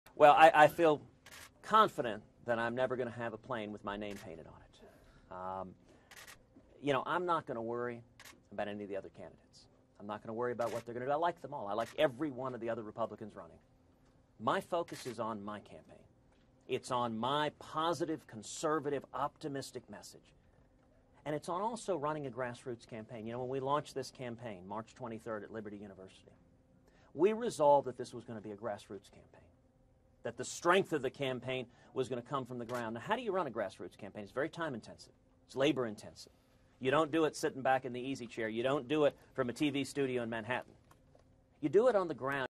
Ted Cruz Says He'll 'Never Have a Plane' With His Name On It
Cruz made the comments during a media gaggle in Iowa.